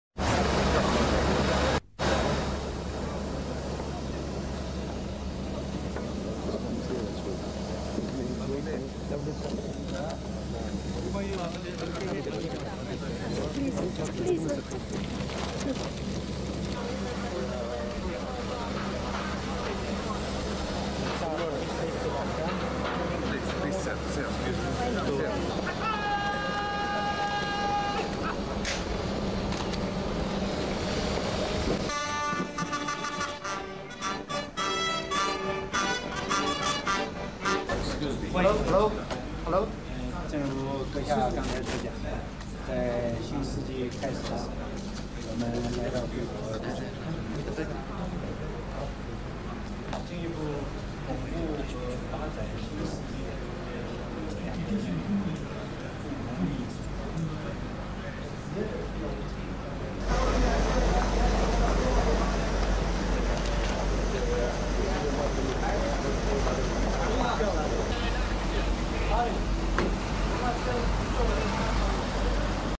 2001年1月，胡锦涛副主席与约旦国王就推动中东和平进程和争取解除对伊拉克的制裁等问题交换了意见，一致表示愿加强协调配合，以求得有关问题尽早得到公正、合理的解决。会见后，阿卜杜拉二世授予胡一级复兴勋章
宾主就推动中东和平进程和争取解除对伊拉克的制裁等问题交换了意见，一致表示愿加强协调配合，以求得有关问题尽早得到公正、合理的解决。会见后，阿卜杜拉二世授予胡锦涛一级复兴勋章，以感谢他为发展约中友谊所作的贡献。

阿卜杜拉二世和王后拉妮亚亚辛还设家宴款待胡锦涛夫妇及主要随行人员。